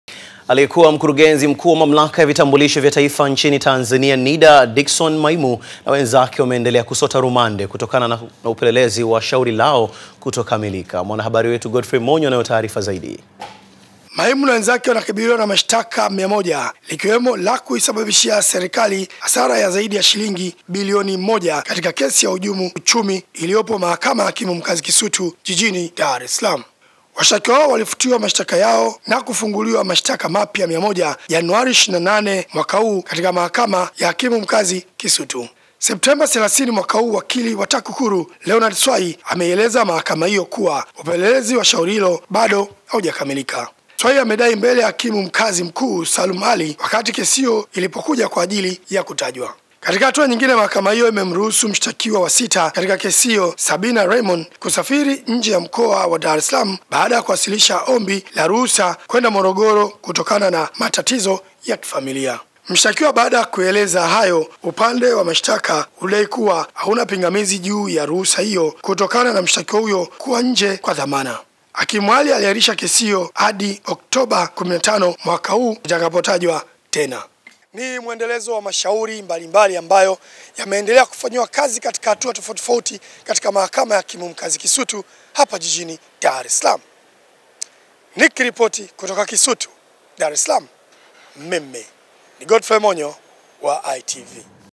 Aliyekuwa Mkurugenzi Mkuu wa (NIDA), Dickson Maimu na wenzake , wanaendelea kusota rumande.
Aliyekuwa Mkurugenzi Mkuu wa Mamlaka ya Vitambulisho vya Taifa nchini Tanzania (NIDA), Dickson Maimu na wenzake , wanaendelea kusota rumande kutokana na  upelelezi wa shauri  lao kutokamilika.